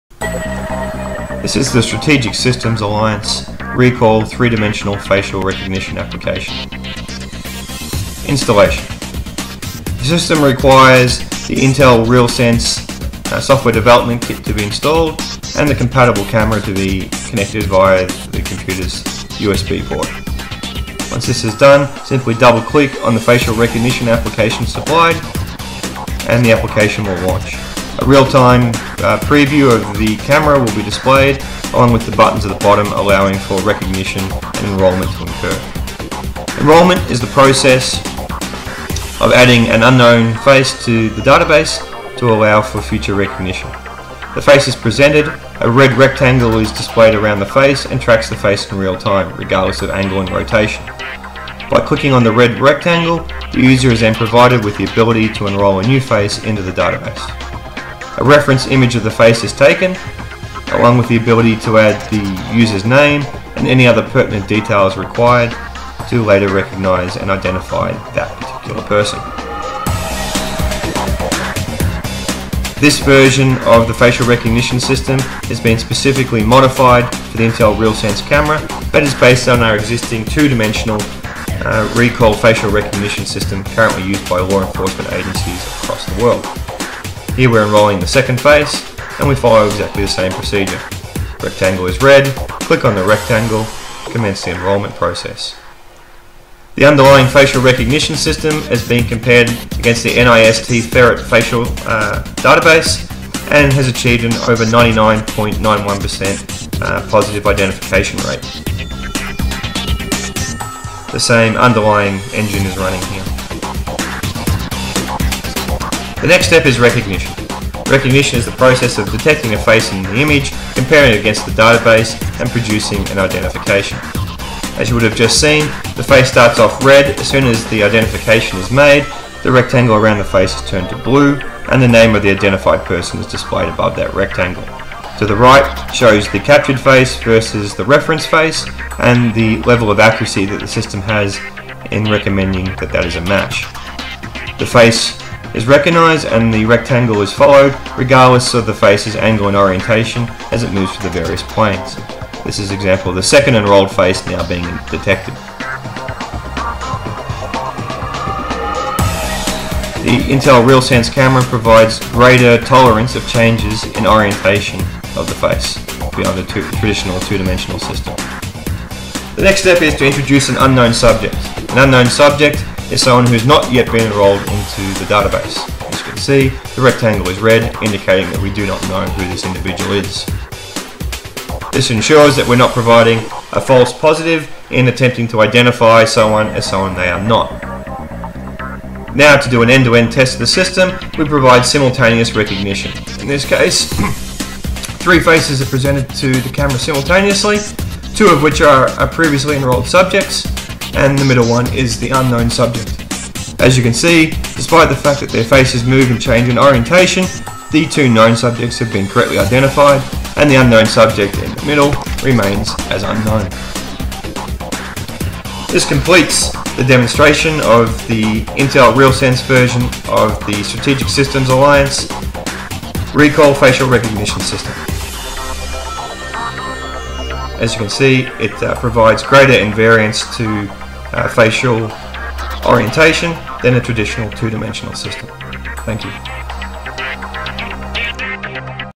SSA Recall 3D Facial Recognition Demo
Intel Real Sense 3D camera variant of the SSA Recall Facial Recognition System